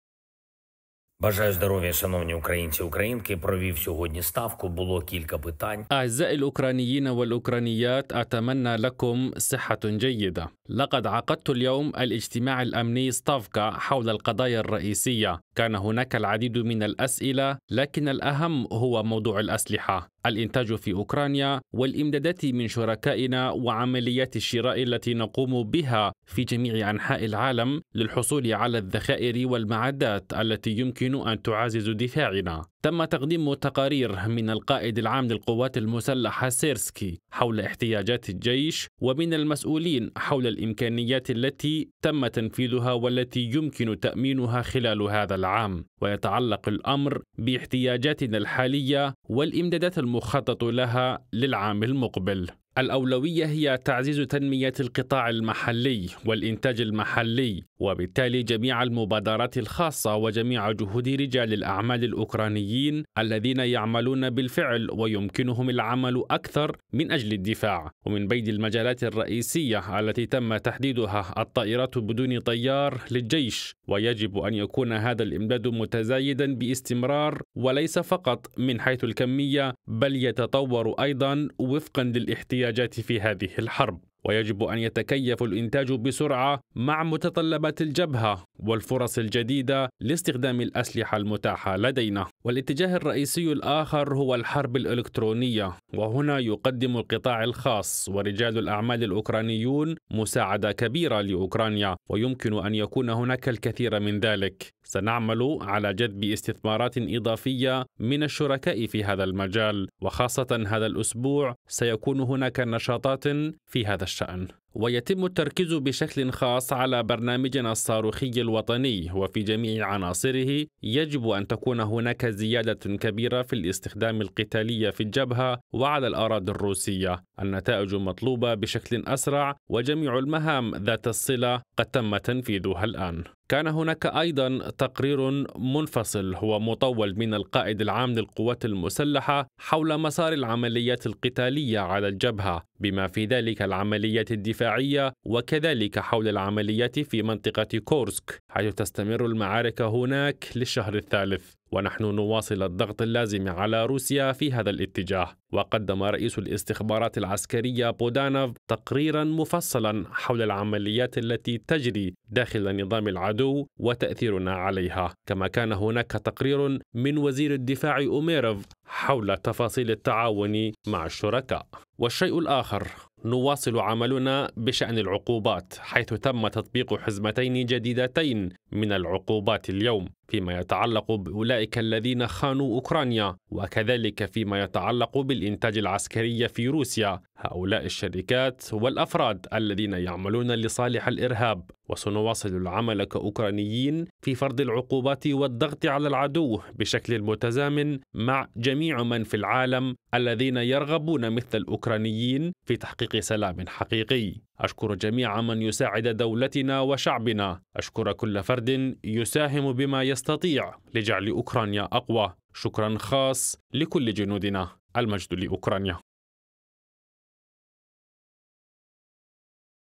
عقوبات جديدة ضد الإنتاج العسكري الروسي والمتعاونين معها
عقوبات جديدة ضد الإنتاج العسكري الروسي والمتعاونين معها ، حيث تم تطبيق حزمتين جديدتين من العقوبات، شملت أولئك الذين خانوا أوكرانيا، وكذلك الإنتاج العسكري في روسيا والشركات والأفراد الذين يتعاملون مع روسيا